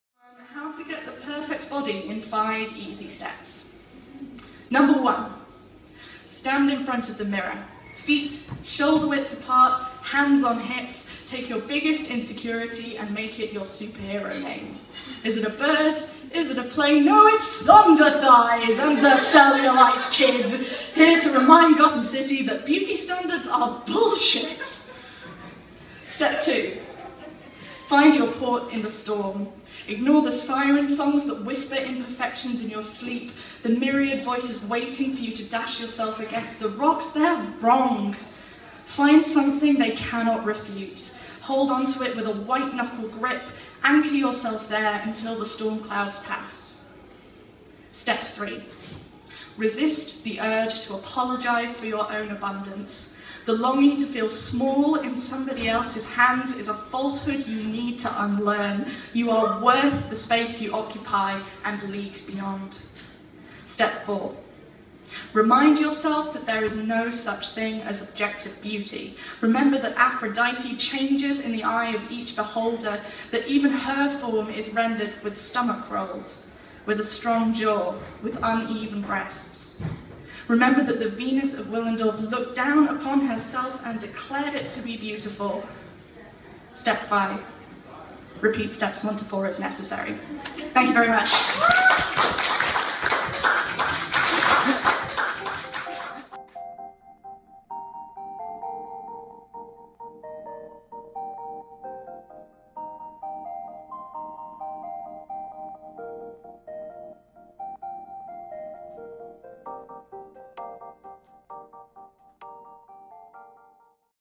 Poem | How To Get The Perfect Body (In 5 Easy Steps)
From my set at the "Patti Cake$" movie launch event at the Showroom Cinema in Sheffield.